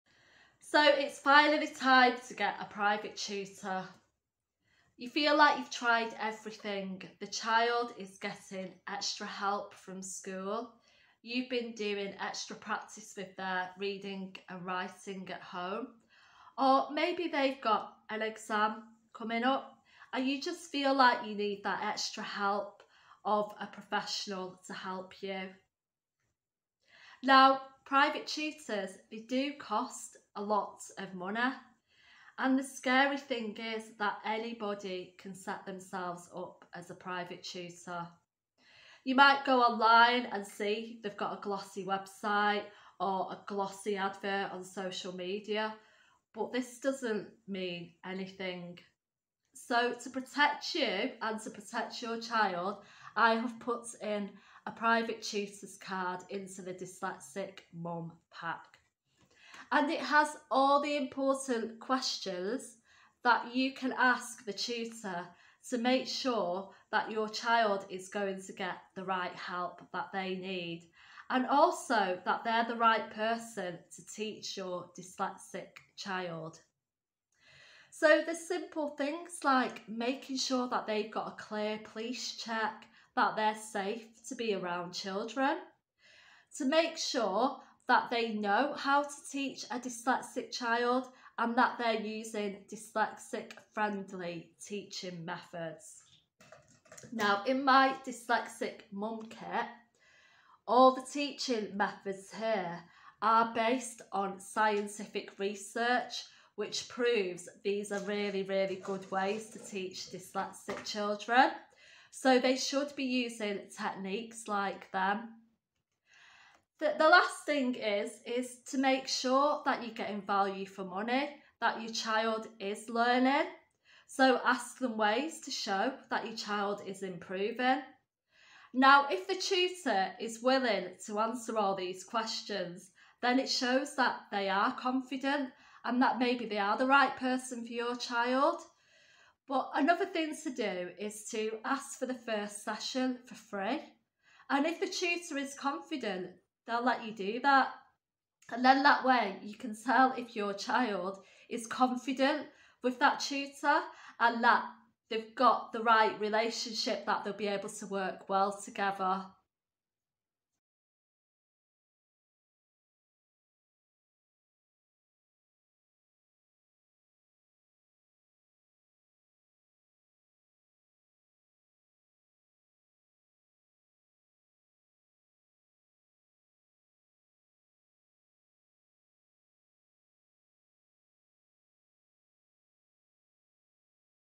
Ask Private Tutors Dyslexic Kids | How to help a Dyslexic Child
Dyslexic children may benefit from having a private tutor. Find out the questions to ask to ensure you get a good dyslexia tutor.